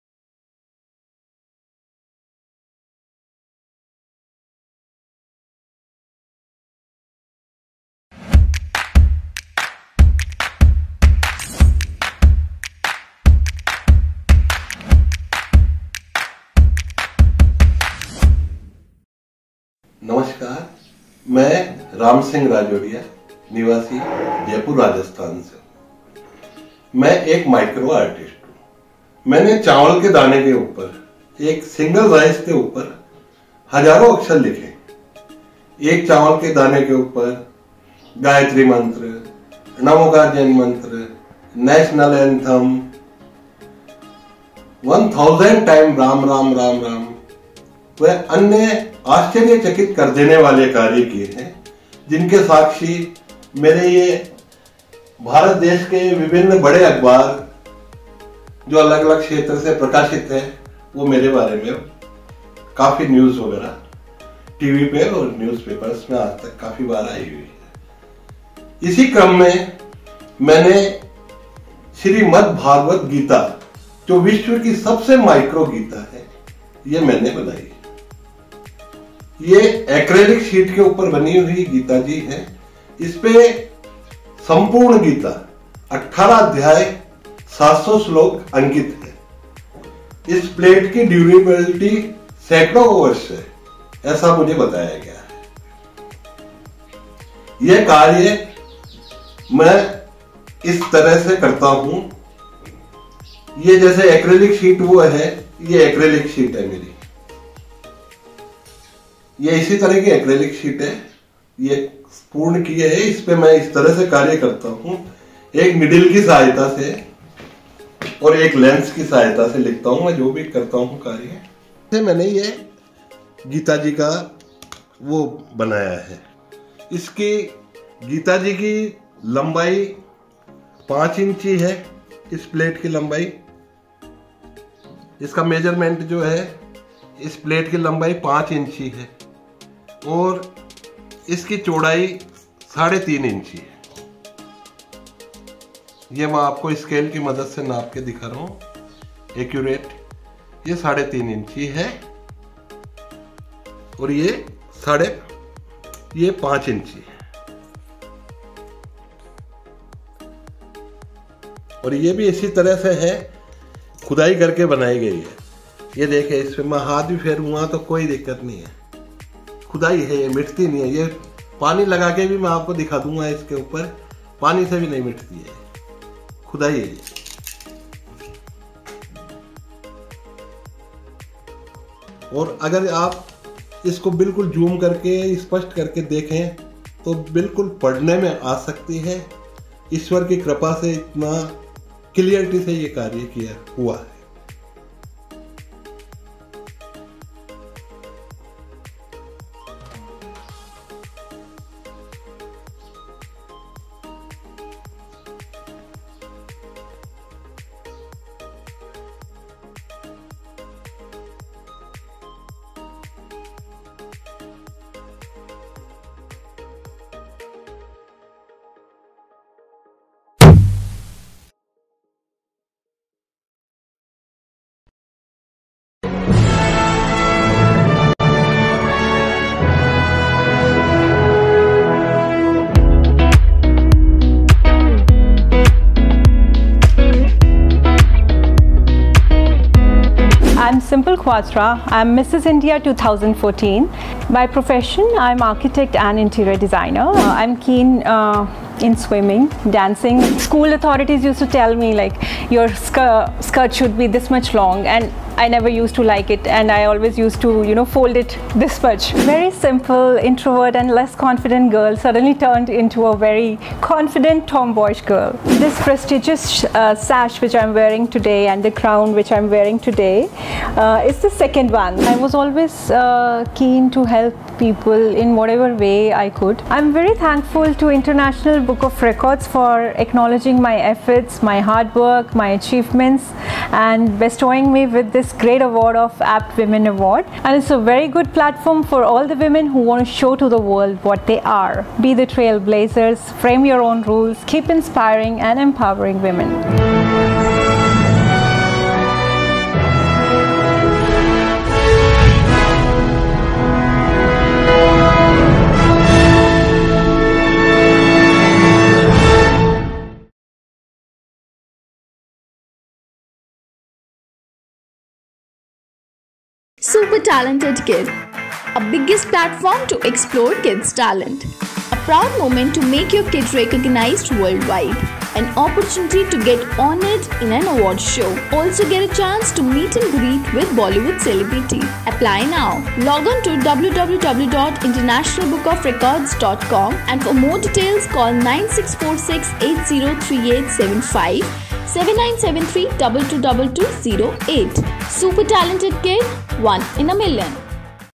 SMALLEST SIZED SRIMAD BHAGAWAD GITA CARVED ON ACRYLIC SHEET
The World Record of  “SMALLEST SIZED SRIMAD BHAGAWAD GITA CARVED ON ACRYLIC SHEET” is achieved by RAM SINGH RAJORIA on 11th February 2022 from Jaipur (Rajasthan) India. He carved 18 chapters and 700 shlokas of Srimad Bhagawad Gita on an acrylic sheet of size 3.5 x 5 inches and has set  a new world record for International Book of Records.